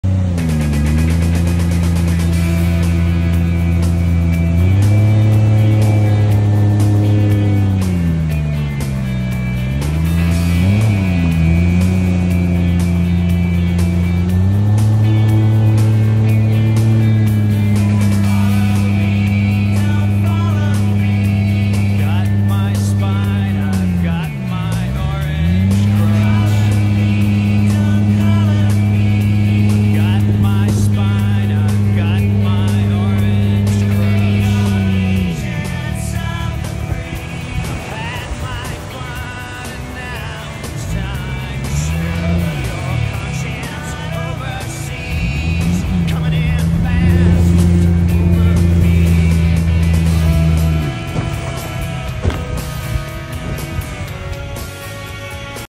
Solar 470 Super Jet On The North Thompson
Just a quick video of me in the rapids on the North Thompson River having some fun.